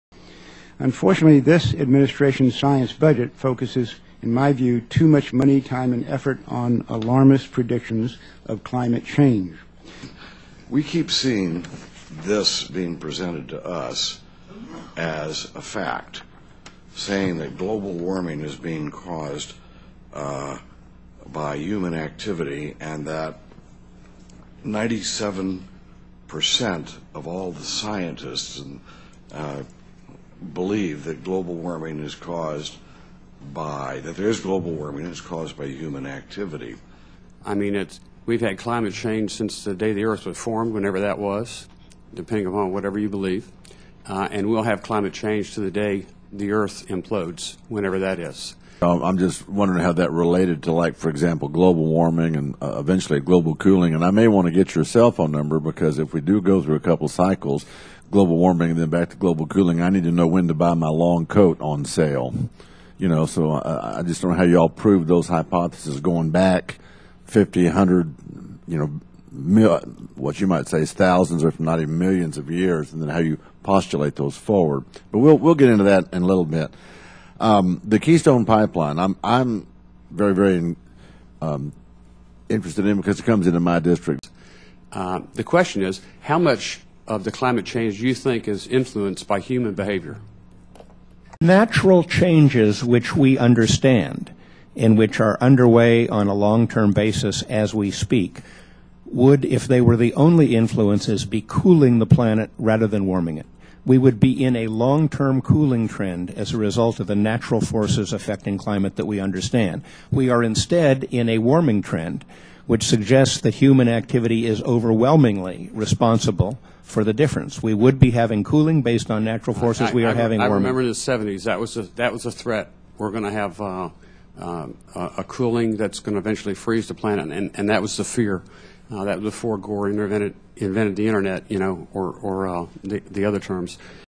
House Science Committee of Climate Denial
3/26/14: The chair, vice chair, and other members of the House Science Committee express contempt for climate science at a hearing with White House Science Advisor John Holdren.